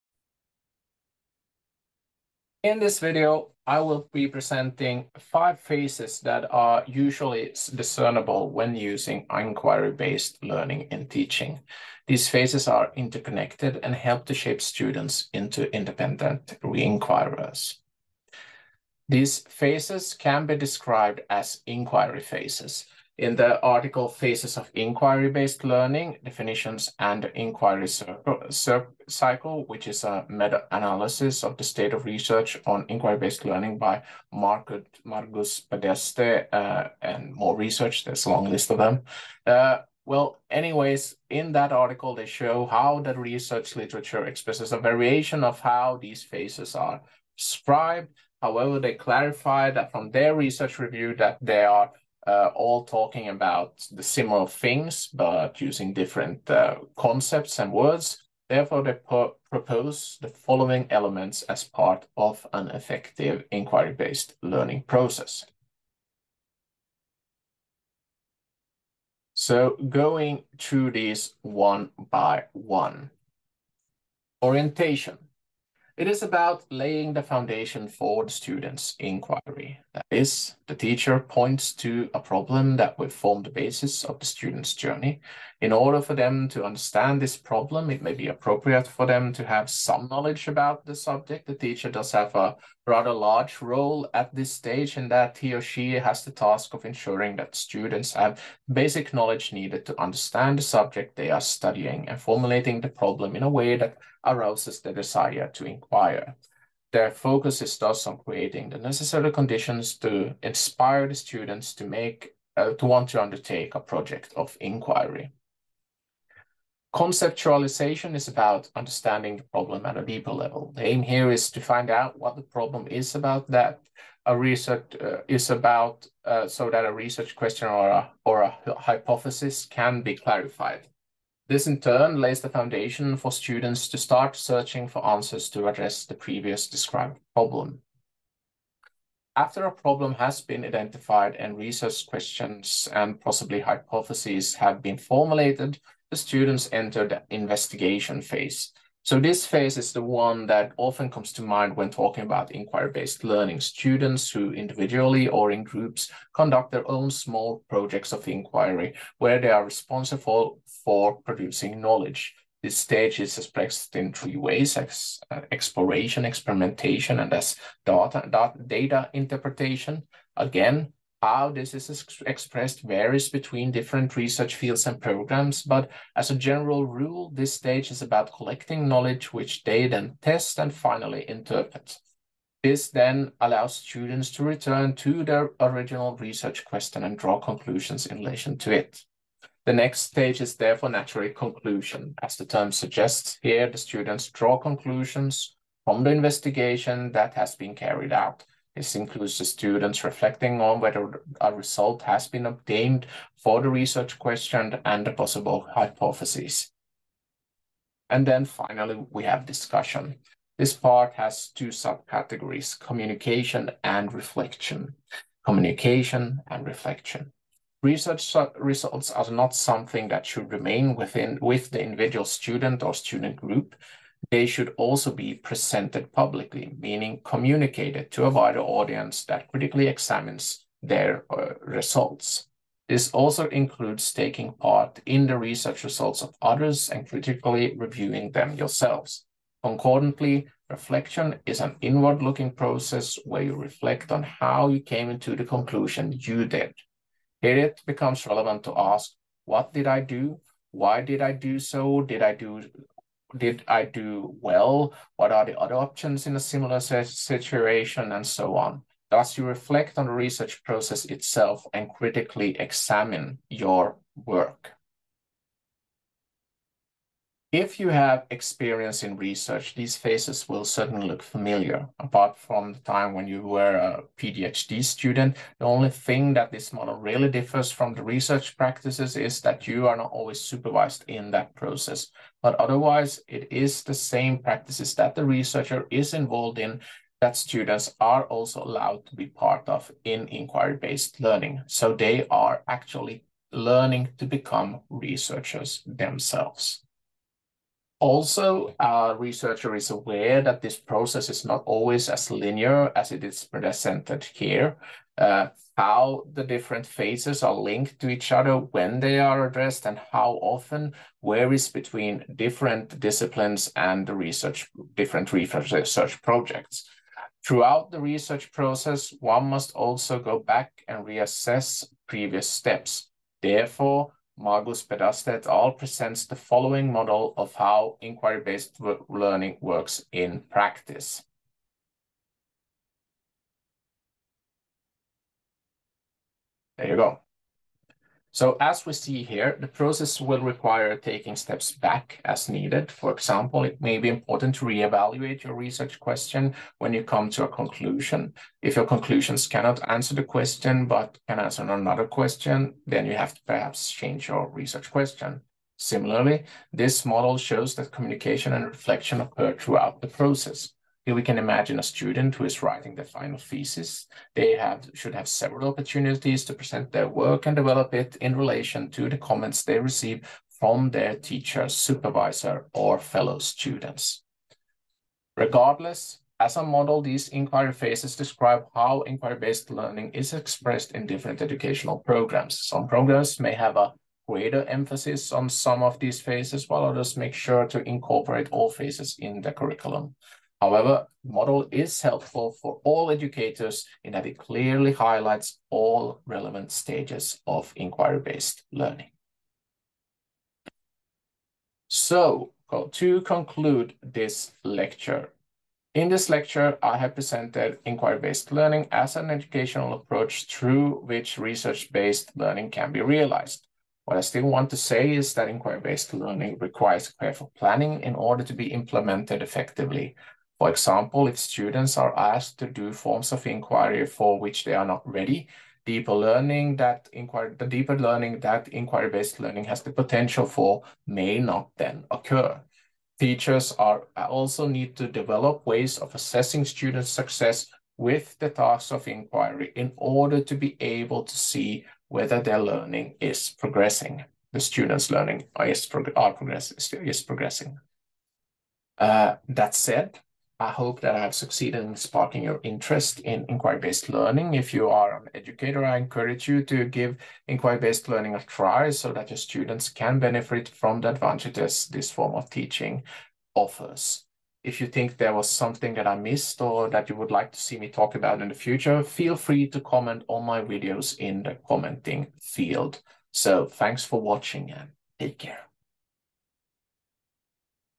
Inquiry phases (5/5)
Title: Inquiry-based learning

In this series of video lectures, we look at inquiry-based learning as a way of enacting research-based teaching and learning. In the video I will be presenting what inquiry-based learning is, how it is generally practices through four forms of inquiry, and how the process towards greater capacity of inquiry goes through five phases. I will also be addressing the question whether all students need to become researchers, which research-based teaching and learning implies is the goal.

(The lecture has been part of a course on research-based teaching and learning in higher education).

Selection of references used in preparation of this lecture:

Banchi, Heather, and Randy Bell. “The Many Levels of Inquiry.” Science and Children 46, no. 2 (October 2008): 26–29.

Cleverly, Dankay. Implementing Inquiry-Based Learning in Nursing. London: Routledge, 2003. doi:10.4324/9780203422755.

Magnussen, Lois, Dianne Ishida, and Joanne Itano. “The Impact of the Use of Inquiry-Based Learning as a Teaching Methodology on the Development of Critical Thinking.” Journal of Nursing Education 39, no. 8 (2000): 360–64.

Moseley, Alice, and John Connolly. “The Use of Inquiry-Based Learning in Public Administration Education: Challenges and Opportunities in the Context of Internationalization.” Teaching Public Administration 39, no. 3 (October 2021): 270–86. doi:10.1177/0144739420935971.

Pedaste, Margus, Mario Mäeots, Leo A. Siiman, Ton De Jong, Siswa A.N. Van Riesen, Ellen T. Kamp, Constantinos C. Manoli, Zacharias C. Zacharia, and Eleftheria Tsourlidaki. “Phases of Inquiry-Based Learning: Definitions and the Inquiry Cycle.” Educational Research Review 14 (February 2015): 47–61. doi:10.1016/j.edurev.2015.02.003.

Spronken‐Smith, Rachel, and Rebecca Walker. “Can Inquiry‐based Learning Strengthen the Links between Teaching and Disciplinary Research?” Studies in Higher Education 35, no. 6 (September 2010): 723–40. doi:10.1080/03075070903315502.